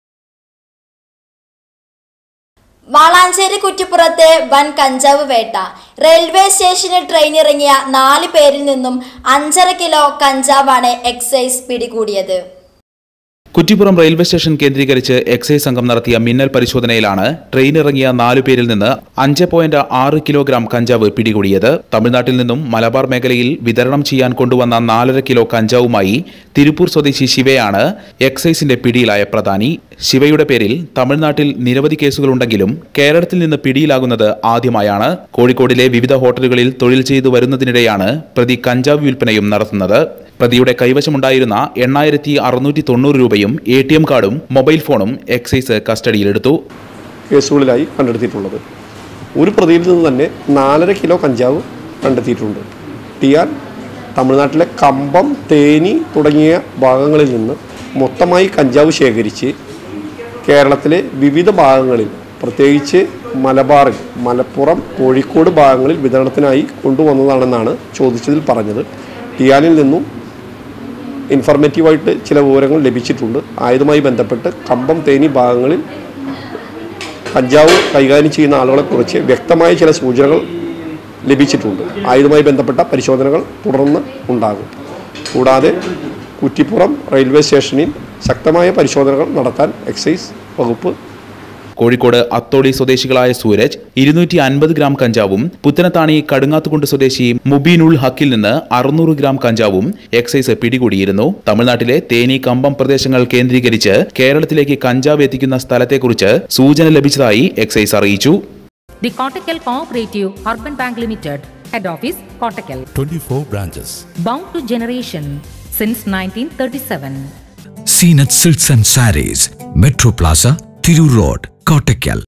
കുറ്റിപ്പുറത്ത് വന്‍ കഞ്ചാവ് വേട്ട - Kanjav - Kuttippuram
കുറ്റിപ്പുറത്ത് വന്‍ കഞ്ചാവ് വേട്ട...റെയില്‍വേ സ്റ്റേഷനില്‍ ട്രെയിനിറങ്ങിയ 4 പേരില്‍ നിന്നും 5 കിലോ 600 ഗ്രാം കഞ്ചാവാണ് എക്‌സൈസ് പിടികൂടിയത്...